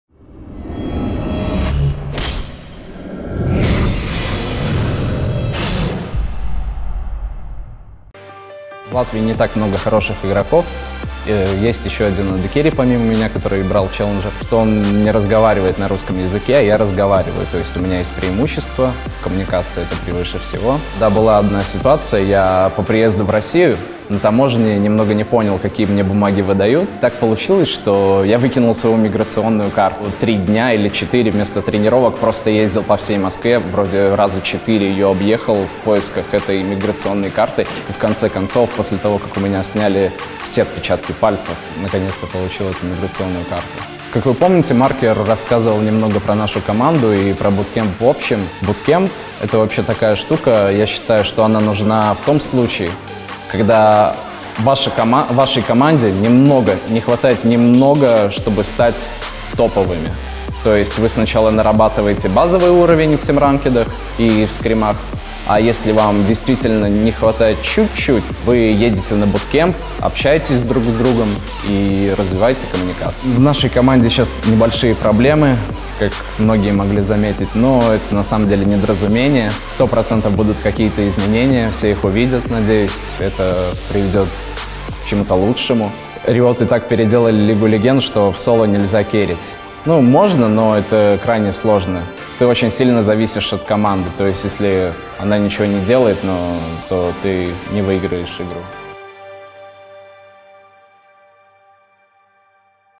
Интервью с JST Blasting
Игрок команды Just.MSI о важности командных действий, неосторожном обращении с документами и роли буткемпа в подготовке команды.

Все новости Континентальной лиги (LCL), включая расписание, результаты матчей и др., доступны на САЙТЕ: